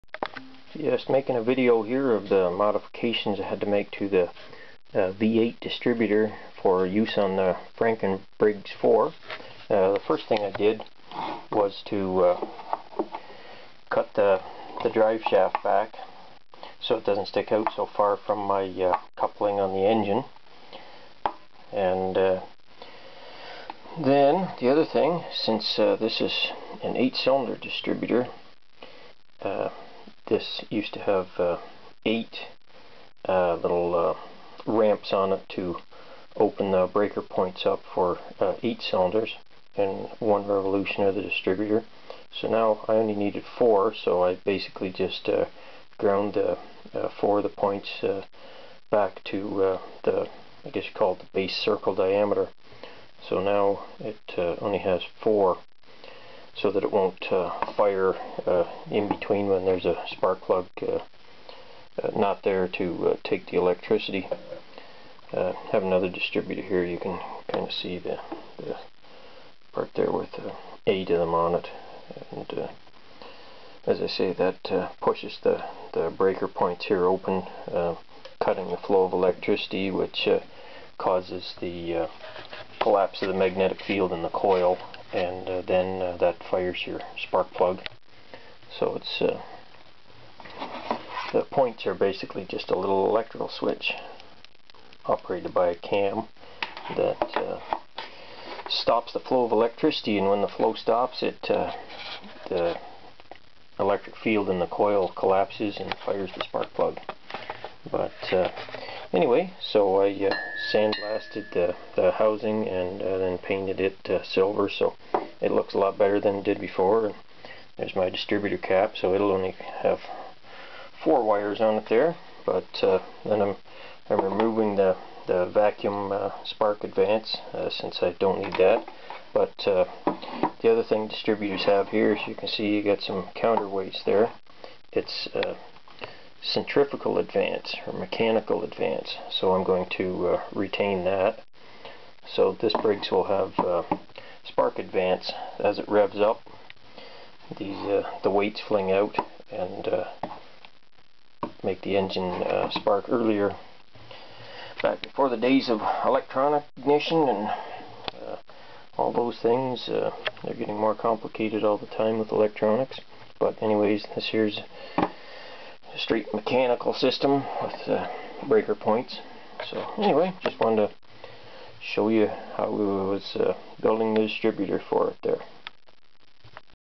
Briggs 4 Distributor
The distributor from a Dodge V8, modified to operate as a 4 cylinder unit.
As shown in the vid, this is a points type distributor (no electronics required).